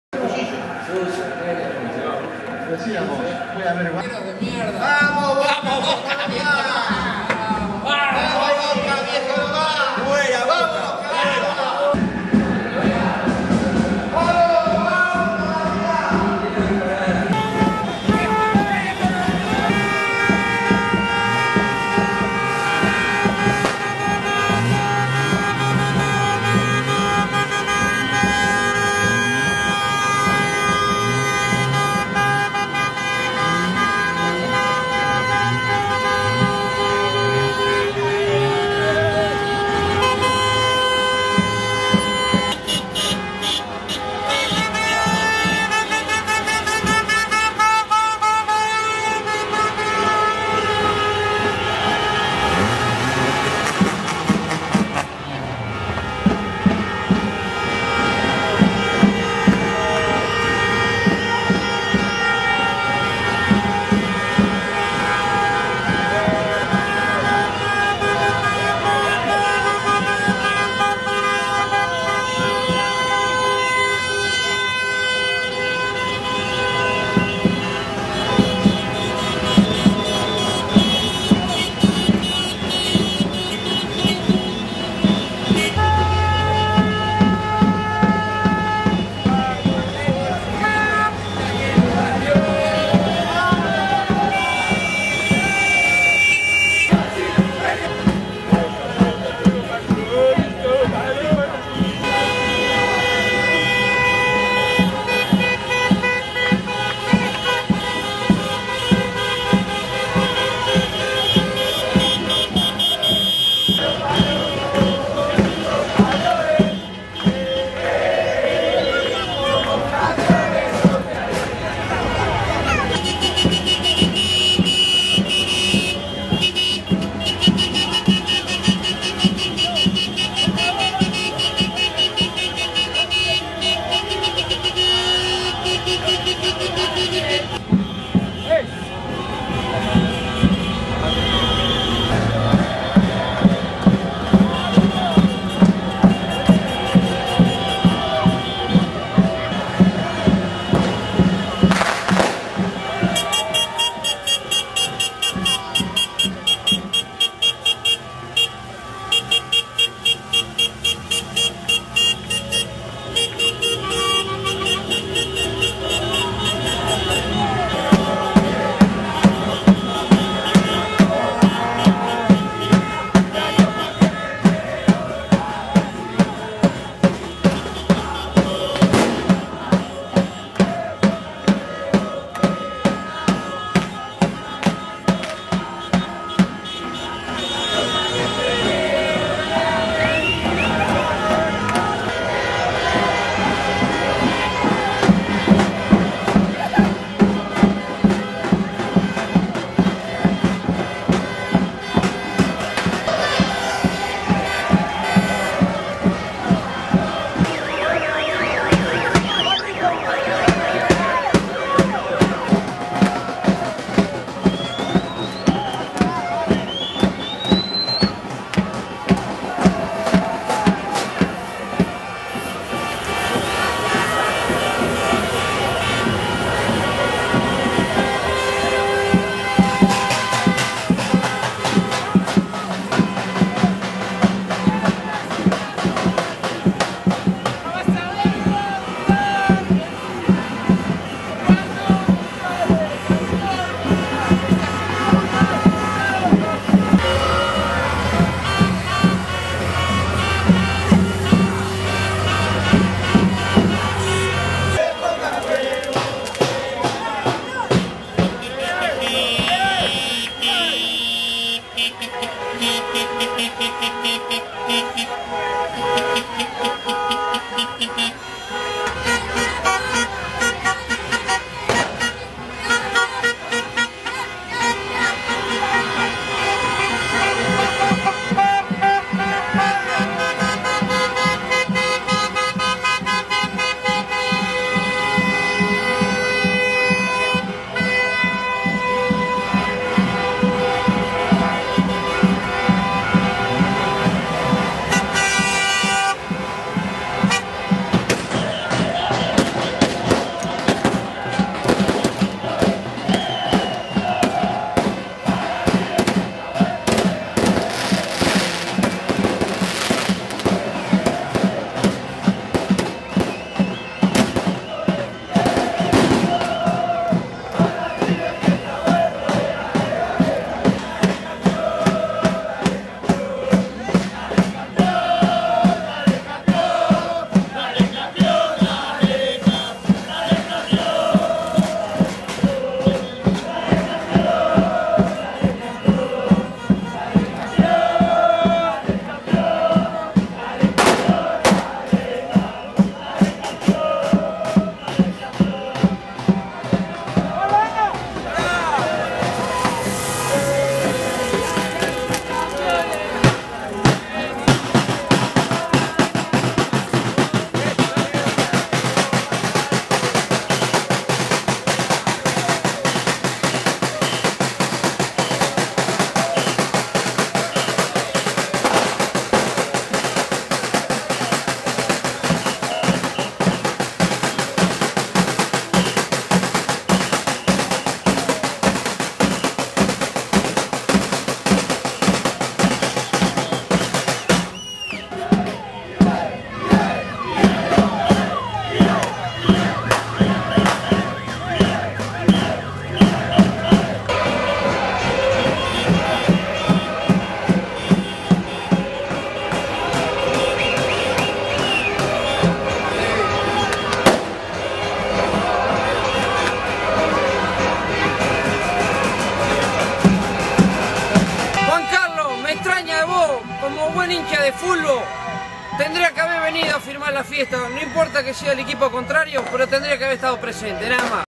FESTEJOS
FESTEJOS DE LA HINCHADA DE BOCA EN CAPITAN SARMIENTO